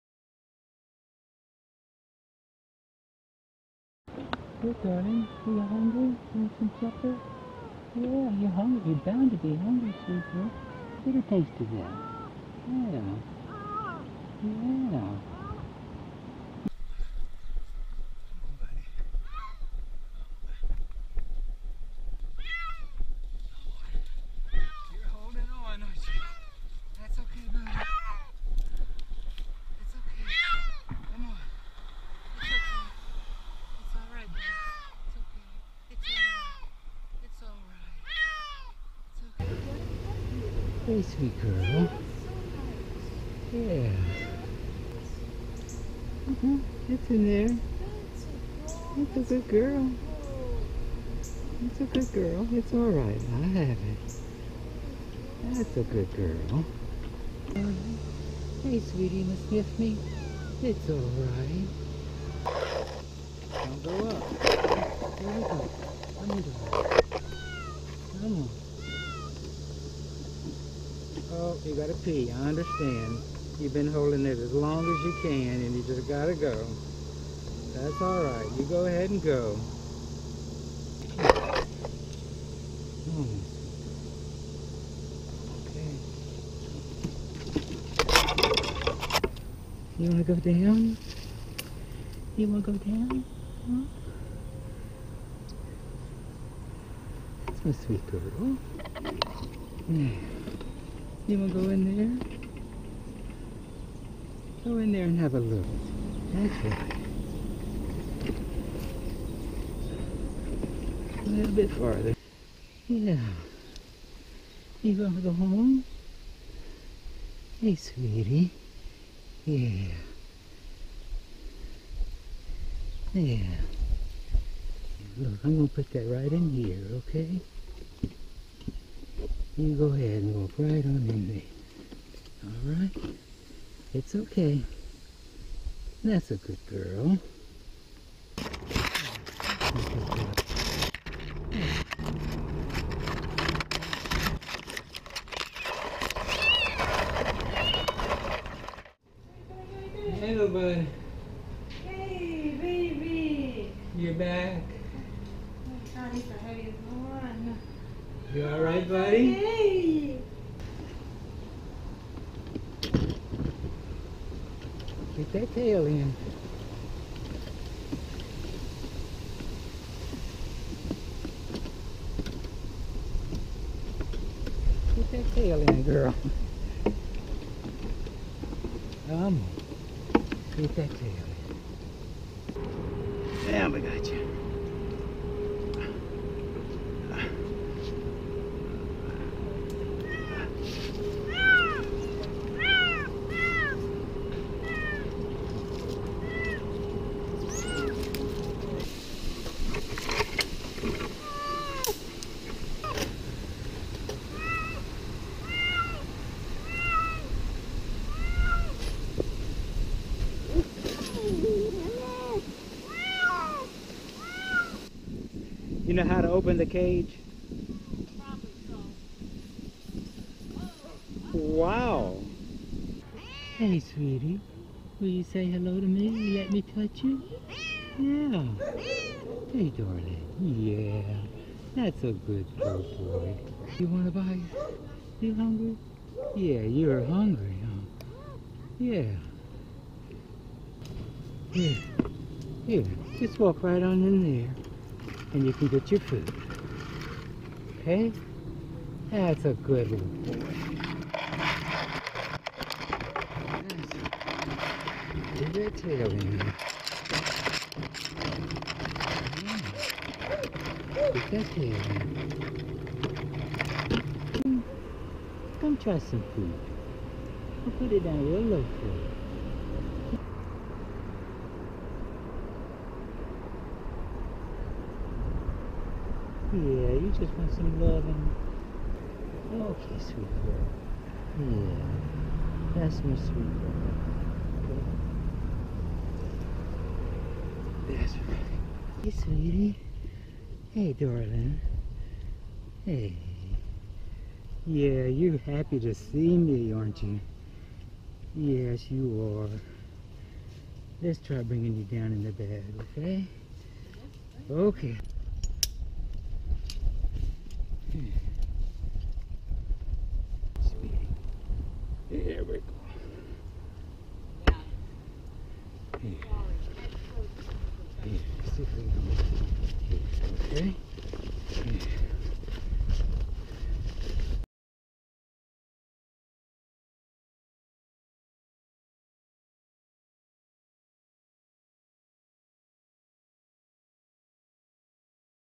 Rescue Highlights of 2015
Highlights of my treetop cat rescues of 2015, beginning with Bella, who was so excited to have food to eat that she vocalized while eating, and ending with Jem, who was so happy to see me that she stepped onto my lap and crawled up on my shoulder.